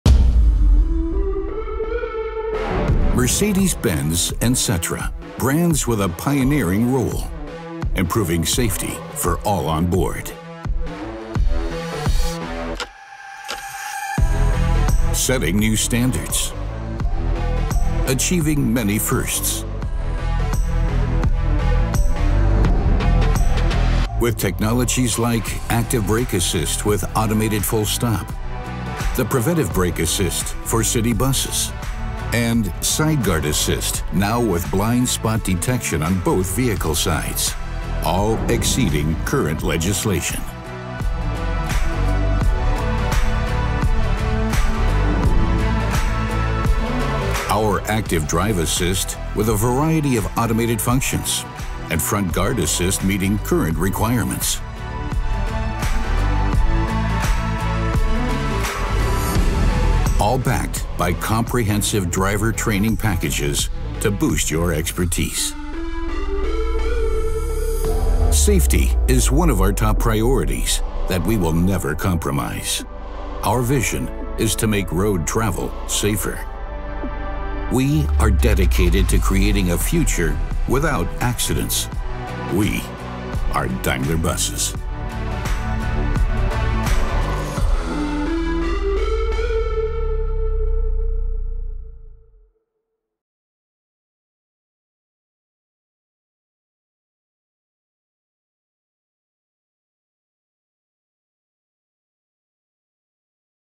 Our way to maximum safety
The safety of drivers and passengers is a top priority at Daimler Buses.
With our numerous assistance systems, such as Blind Spot Assist or Active Brake Assist, we support you in daily road traffic.
It is our ambition to make the mobility of tomorrow not only emission-free, but also as safe as possible.